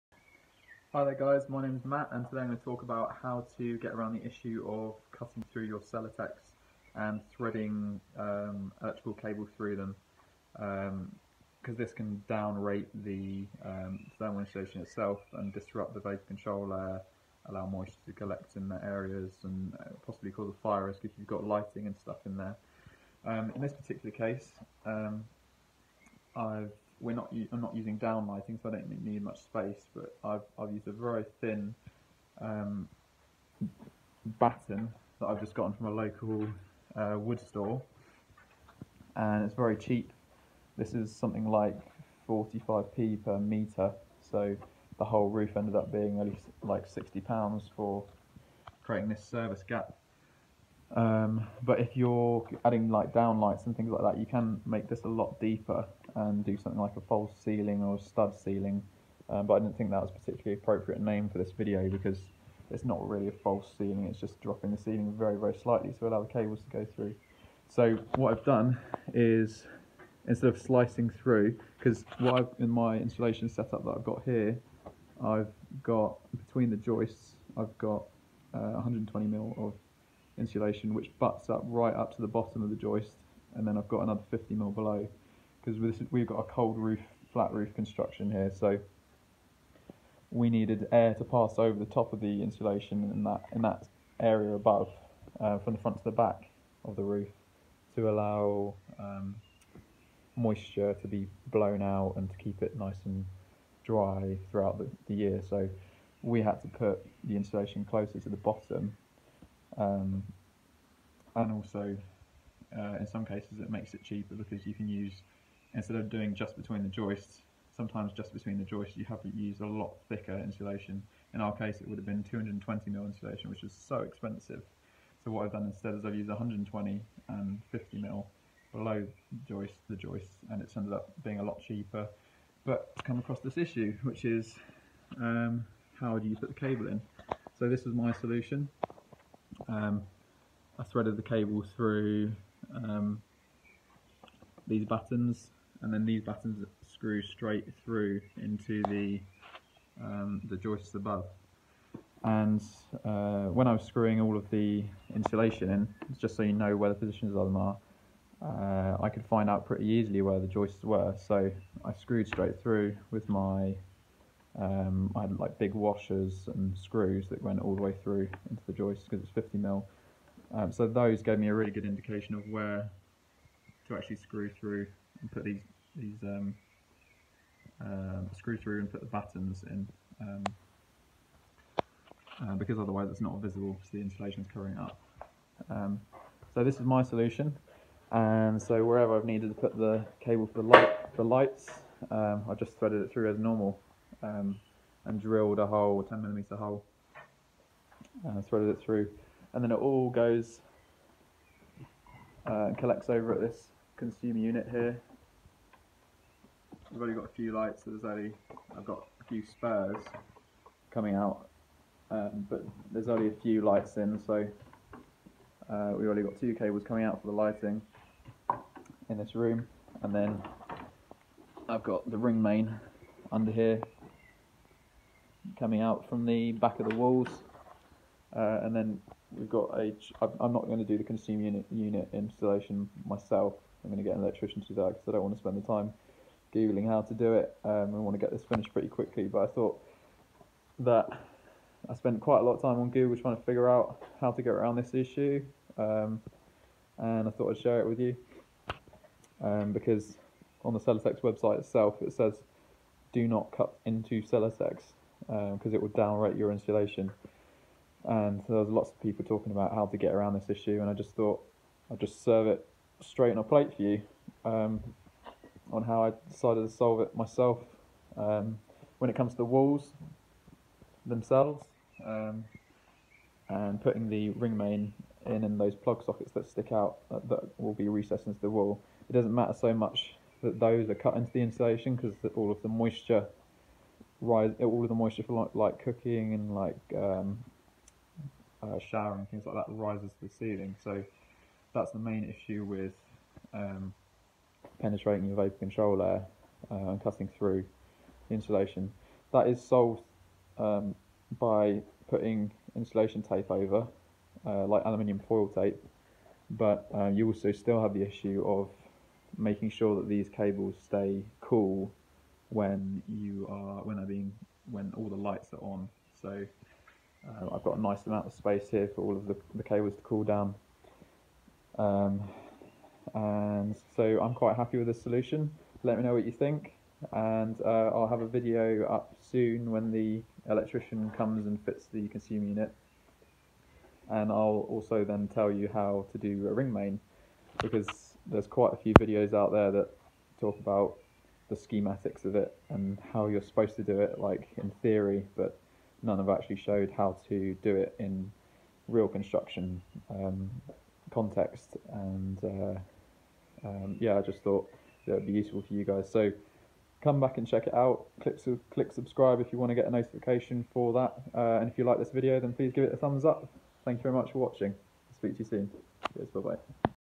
Electrical Cables / Wires Running Through Celotex Kingspan or Ecotherm , What to Do?
This video offers an alternative to slicing channels for wires / electrical cable into your Celotex , Kingspan or ecotherm rigid PIR foam insulation. This problem is often faced when situations when insulation is placed between and also below joists in flat roof constructions.